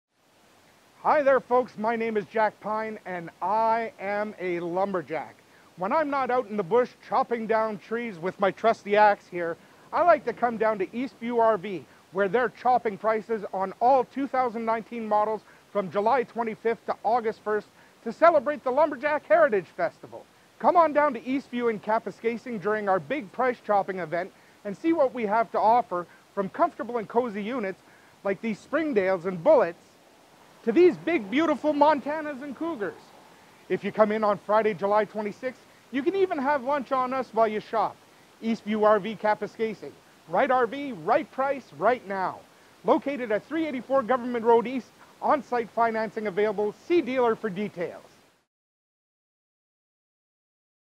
Eastview RV - Lumberjack Festival Sale 2019
Eastview RV's sale during the 2019 Lumberjack Heritage Festival, featuring spokesperson "Jack Pine".

Visit one of Eastview RV's two locations:

Timmins
3449 Hwy. 101 W. (Just past Kamiskotia Rd.)

Kapuskasing
224 Government Rd. E.